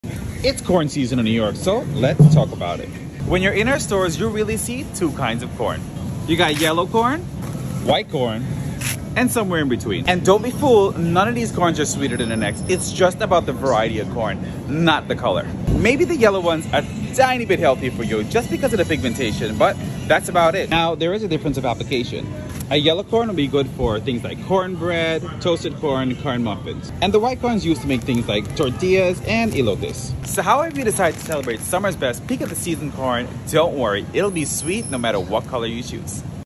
Corn Season | Fairway Market
Have you ever wondered the difference between white and yellow corn? 🌽  Follow along and learn a bit more about corn while it's in season!